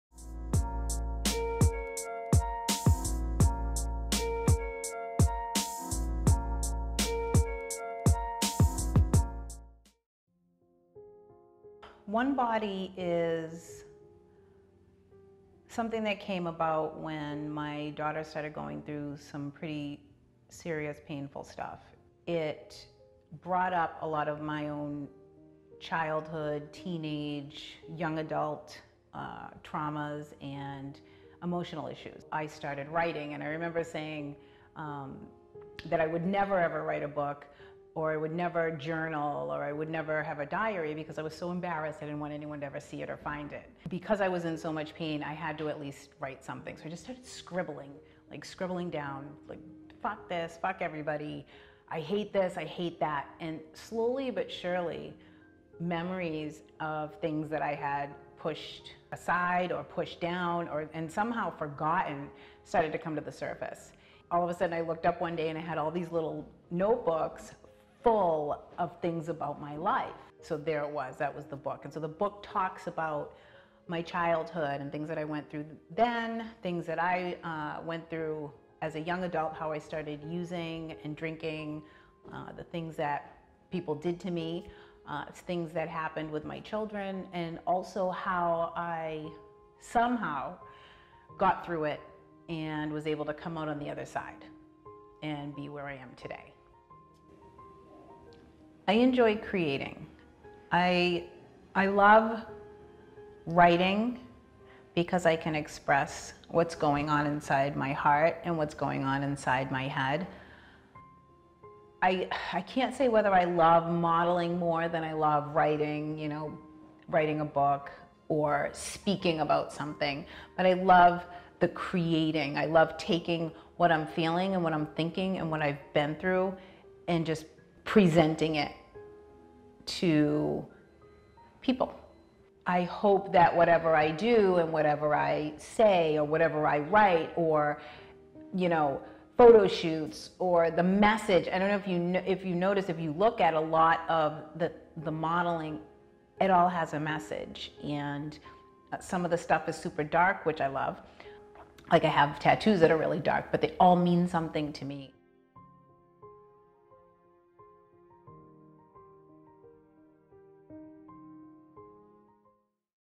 SARAH LOVELL: TRUTH PART 1
Sarah Lovell, Author, Survivor, Motivator talks about  her life, her survival, and her book, One Body in this 5 part interview series to introduce her upcoming web series, The Sarah Lovell Show.

Section 1: One Body
Section 2: Creating

Directed & Edited By SandoFilms
Photos by 781 Photography
Makeup By Misty Lockheart
RPM FITNESS for location